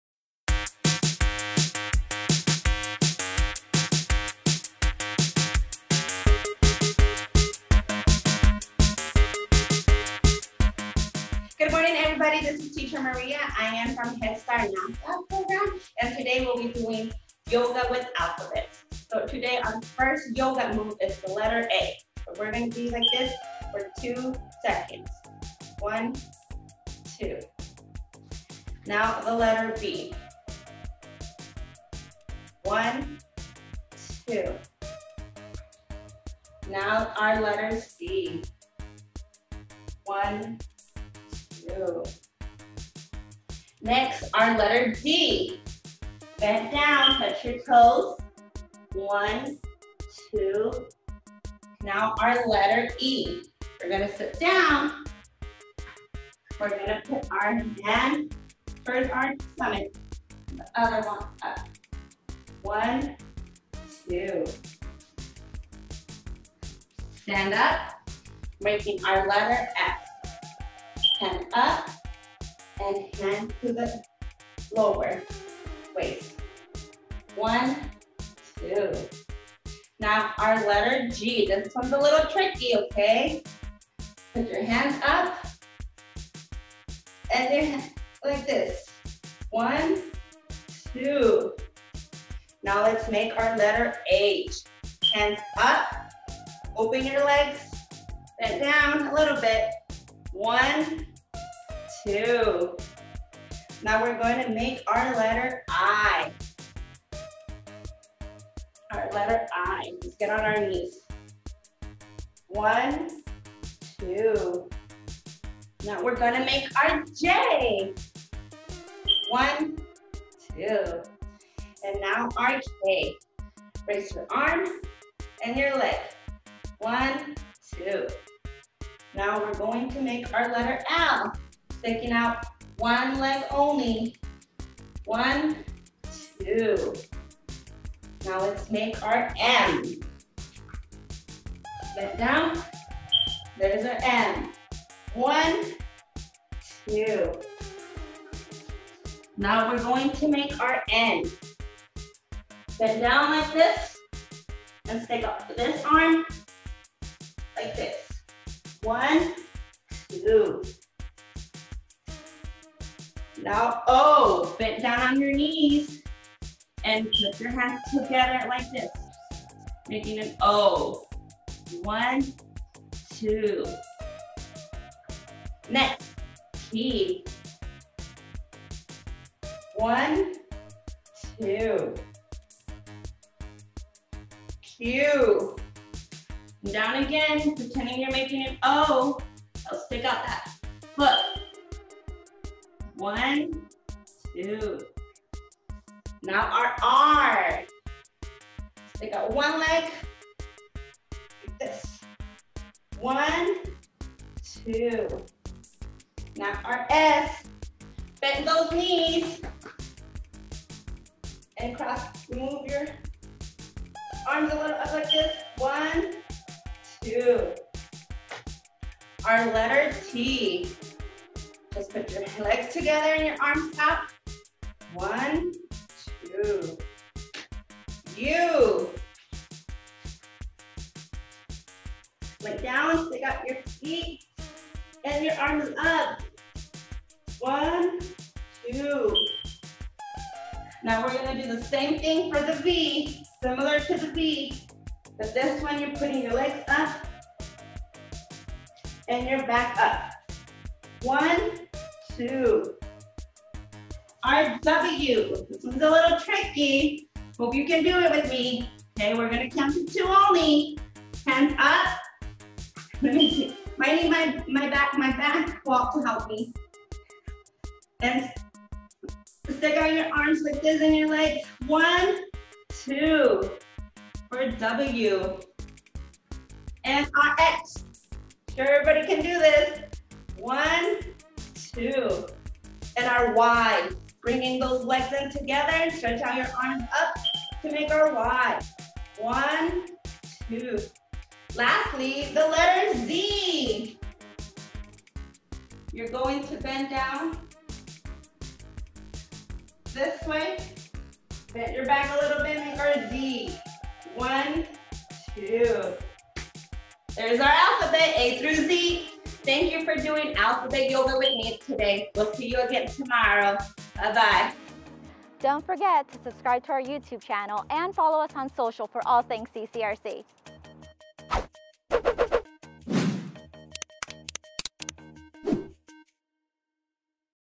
Get Ready for Alphabet Yoga!
Welcome to today’s Head Start at Home yoga activity!

Today we are going to get some exercise in the form of yoga and work on our gross motor skills! Follow along as Teacher Maria teaches us the letters of the alphabet through yoga poses!

Not only are we learning the alphabet but we’re exercising at the same time!

Can you spell out your name with Alphabet Yoga?

We're now enrolling children zero to five! CCRC's award winning Head Start program is accepting applications. to learn more and to apply.